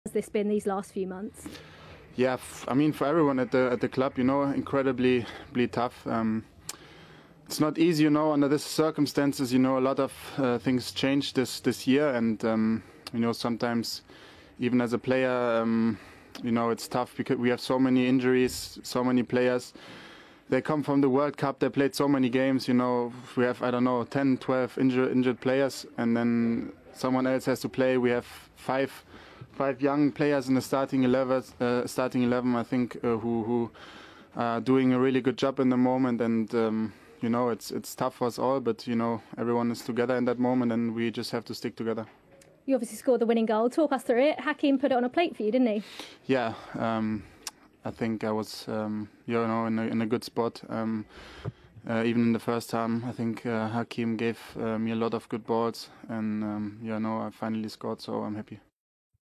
🔥Chelsea 1 | Crystal Palace 0 | Kai Havertz BULLET header wins the game
Kai Havertz scores the winning goal with a bullet header. Chelsea beat Crystal Palace 1 - 0 in the Premier League